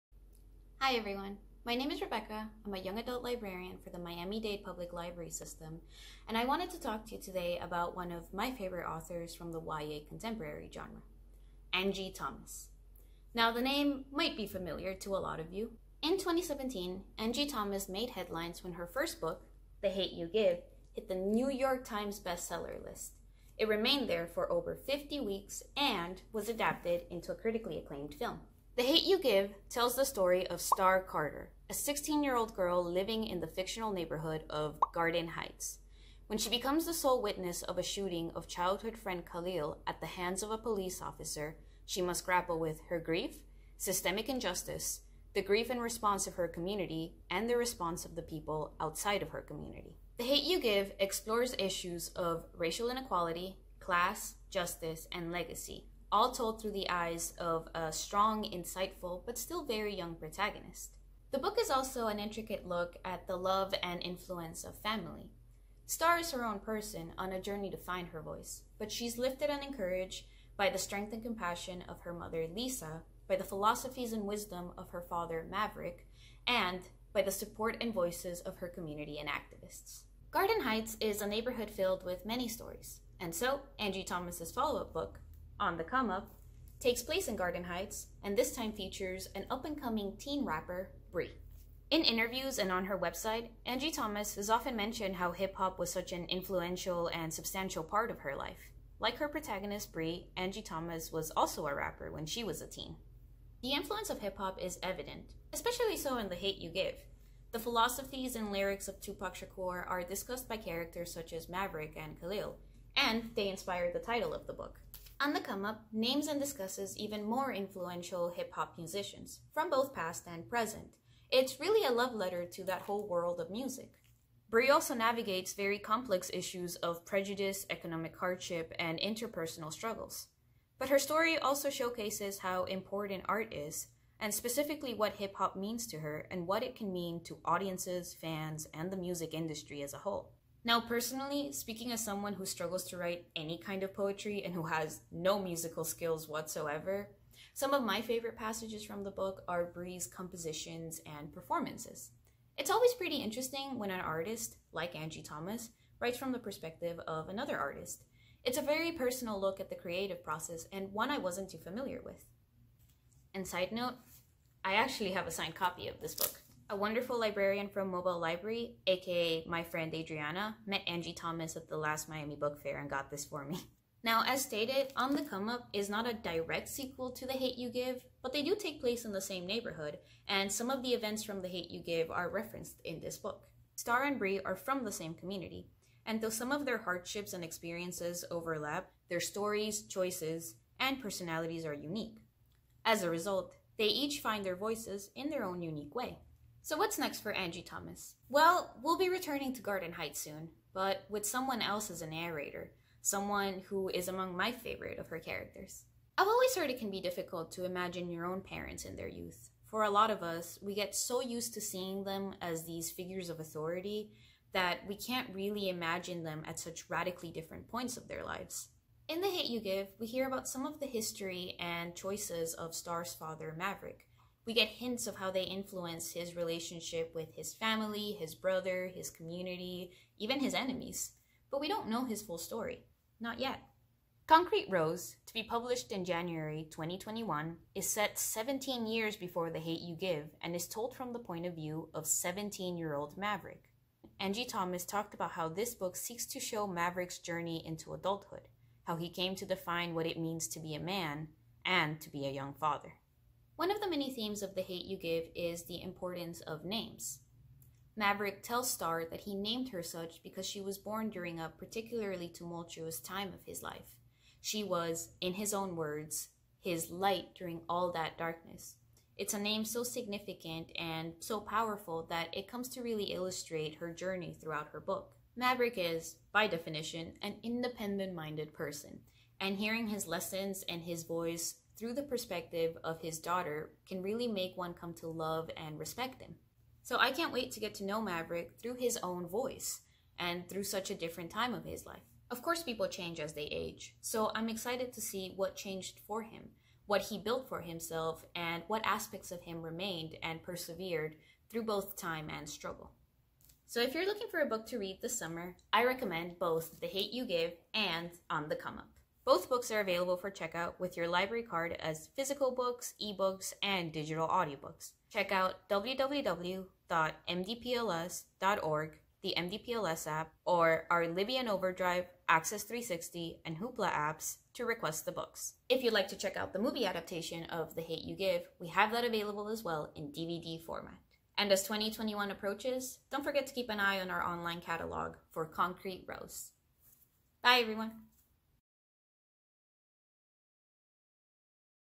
Author Spotlight: Angie Thomas
Have you read the works of bestselling author Angie Thomas? After hearing from MDPLS librarian Rebeca during this author spotlight, you’ll be adding “The Hate U Give” and “On the Come Up” to your reading list!